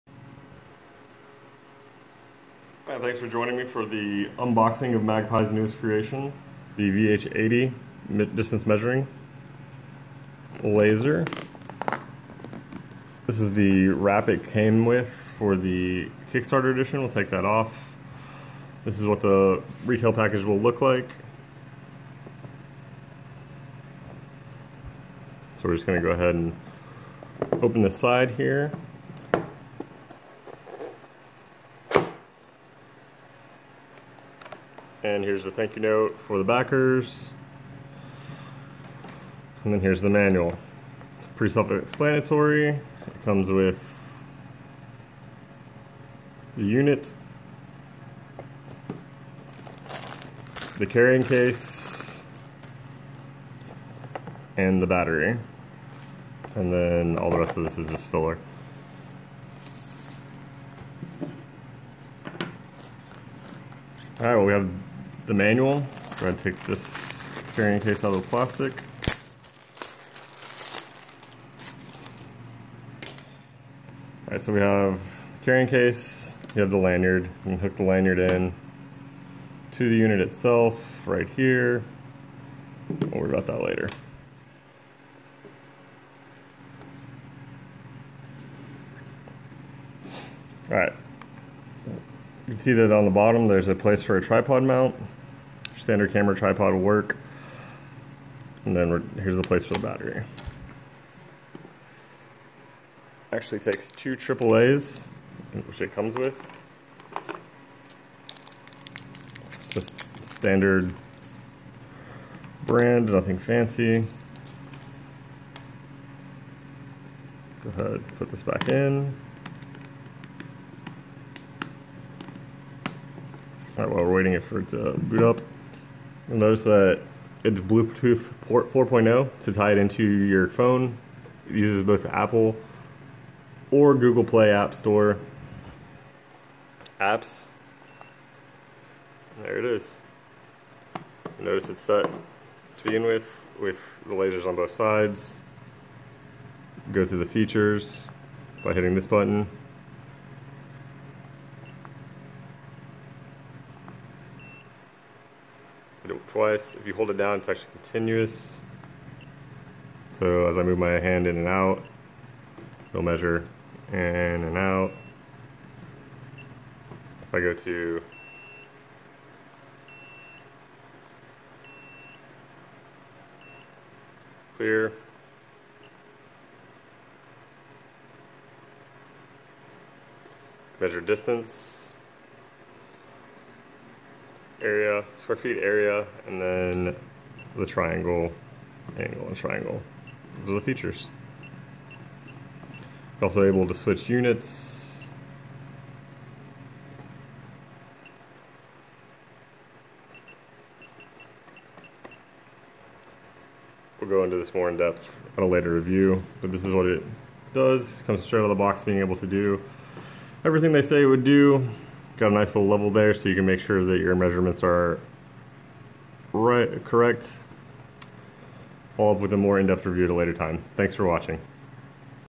Magpie VH-80 Unboxing
Magpie VH-80 Bilateral Laser Distance Measurer Kickstarter Unboxing.